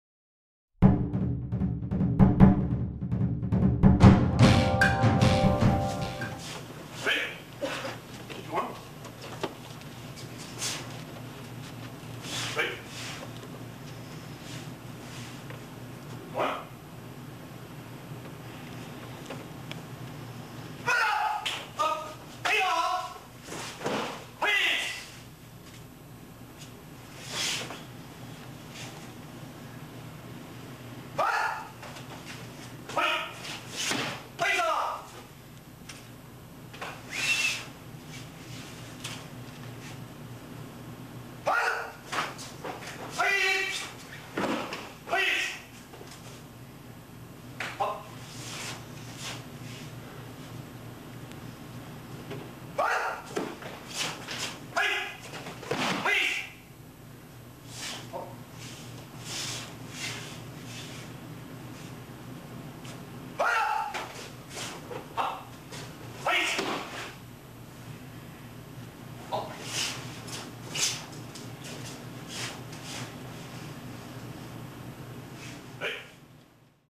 One-Step Striking 一步打拳
Chinese kungfu practice of rudimentary
counter-attack striking. Commonly called
 "one-step" sparring.

Old clip of Mr. Awad and Mr. Stapleton at the
CMAI in 1988.